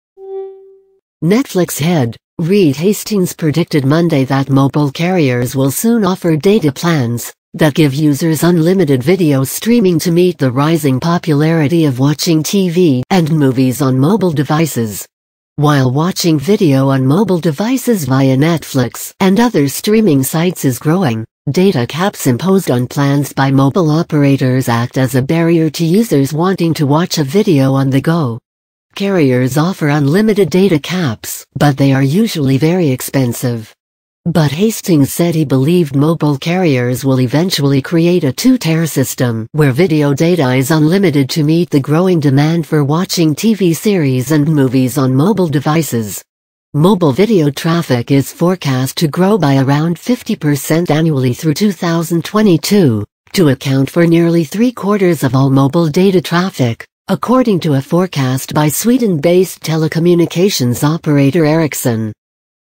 Netflix boss predicts mobile operators will soon offer unlimited video
Netflix head, Reed Hastings predicted Monday that mobile carriers will soon offer data plans. that give users unlimited video streaming to meet the rising popularity of watching TV and movies on mobile devices.

While watching video on mobile devices via Netflix and other streaming sites is growing, data caps imposed on plans by mobile operators act as a barrier to users wanting to watch a video on the go.
Carriers offer unlimited data caps but they are usually very expensive.
But Hastings said he believed mobile carriers will eventually create a two-tear system where video data is unlimited to meet the growing demand for watching TV series and movies on mobile devices.

Mobile video traffic is forecast to grow by around 50 percent annually through 2022, to account for nearly three quarters of all mobile data traffic, according to a forecast by Sweden-based telecommunications operator Ericsson.